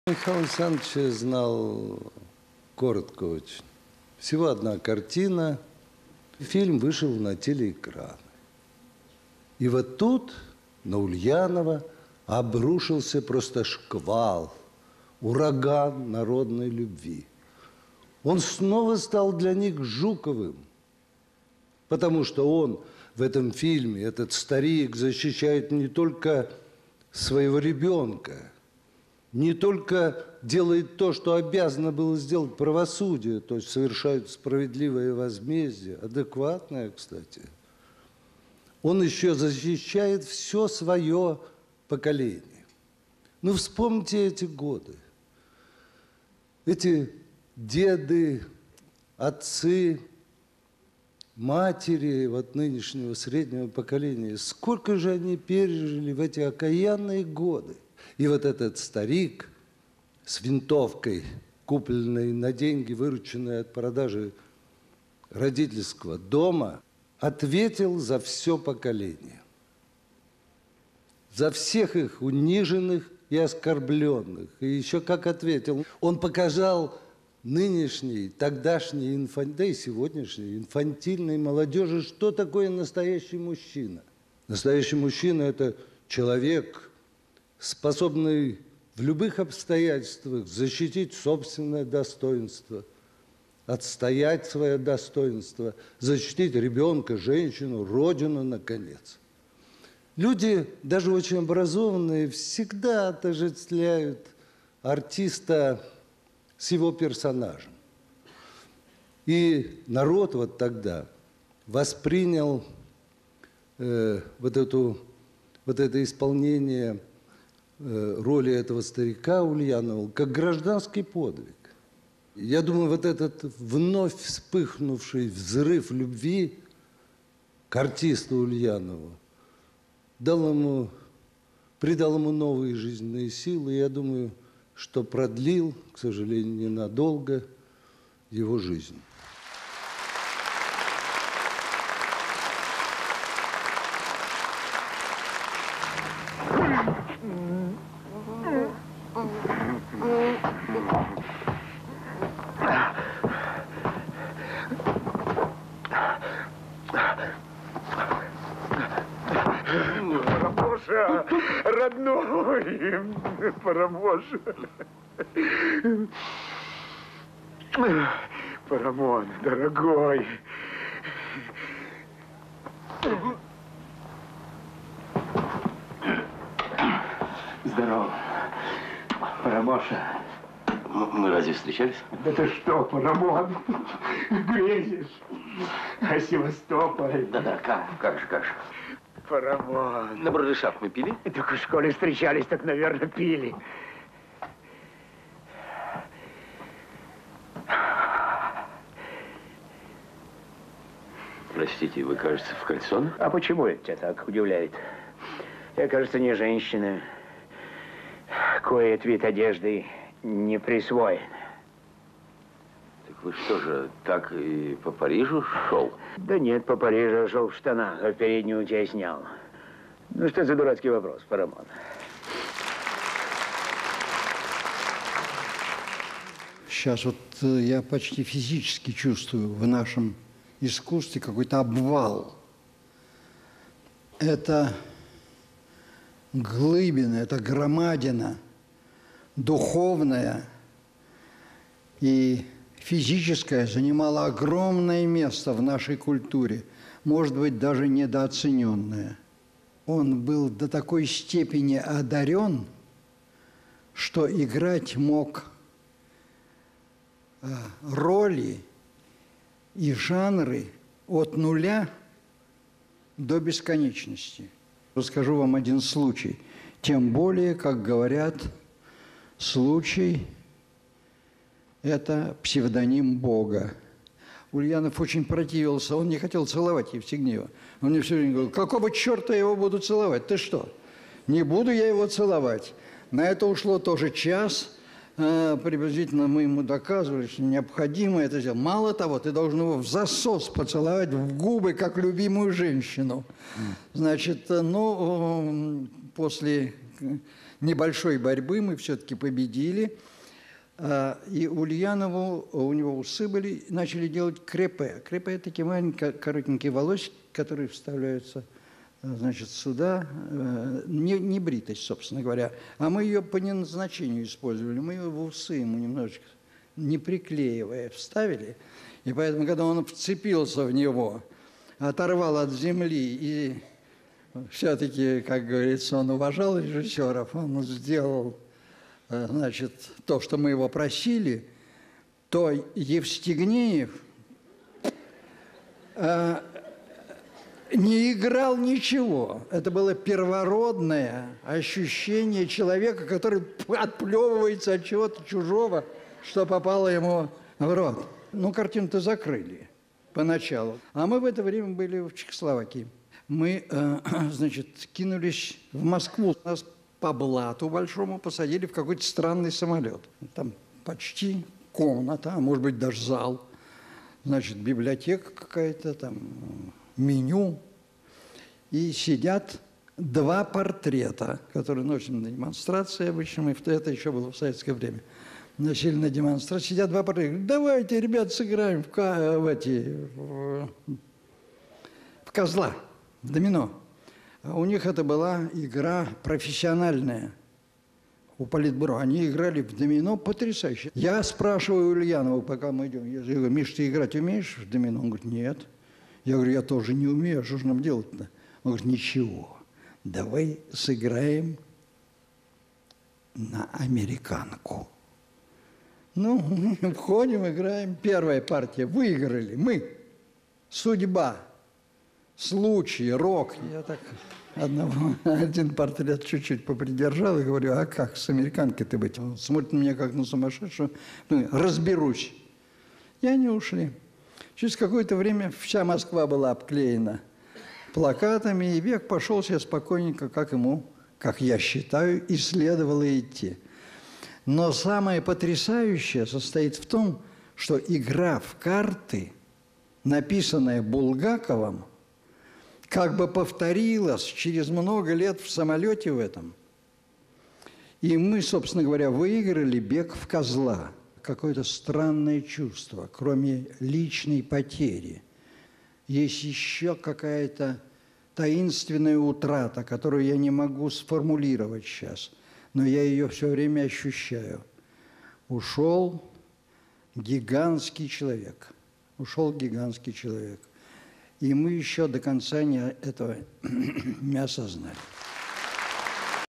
Михаил Ульянов в образе и жизни (5-я часть)
Вечер в ЦДЛ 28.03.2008 О Михаиле Ульянове вспоминают С.Говорухин, В. Наумов